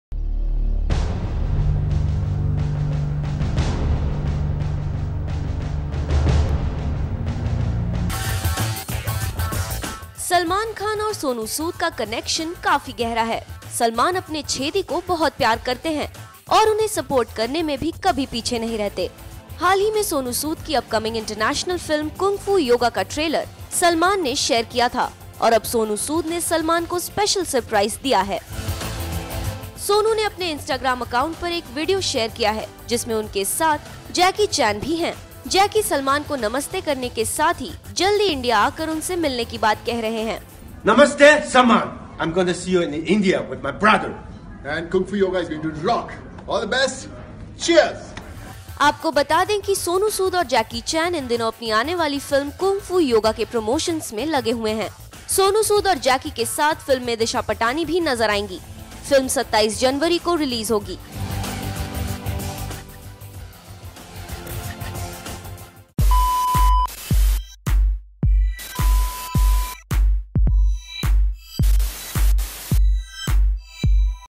Sonu Sood & Jackie Chan, Salman को देने जा रहे हैं Surprise
Jackie Chan has kicked off the promotions of his upcoming film, Kung Fu Yoga in full swing. The film, which also stars Indian actors Sonu Sood, Disha Patani and Amyra Dastur, will also be promoted in India. It’s definitely a great news for Chan’s Indian fans who will get to see the action star flying down to Mumbai. The dates of his visit are yet to be confirmed.